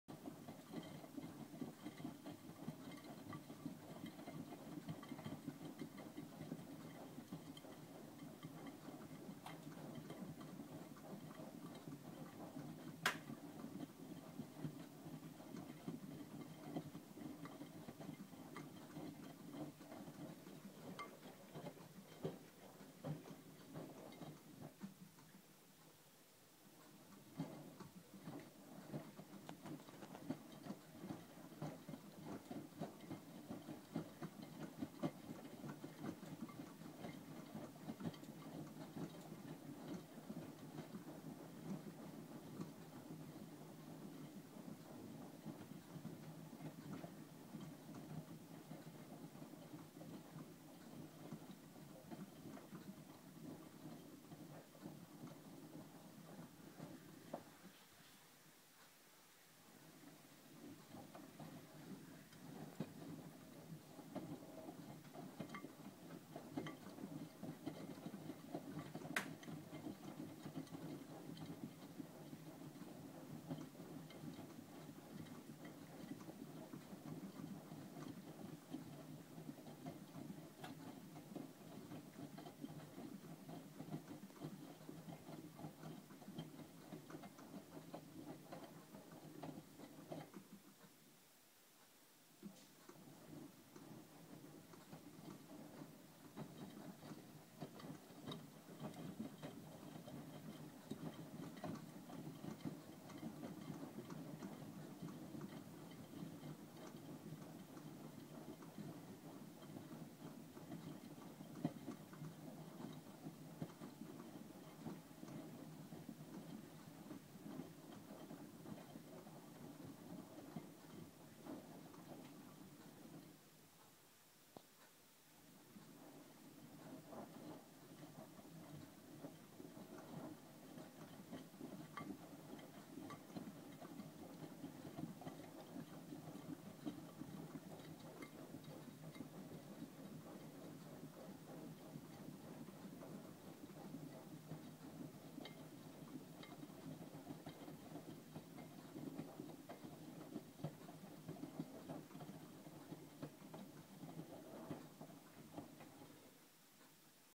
Miele W3922WPS Towels & Bath Mats 75*C Part 2
Heated up and washing away :-) if you listen carefully you can hear the heater switching on and off - making sure the temperature stays at 75 :-)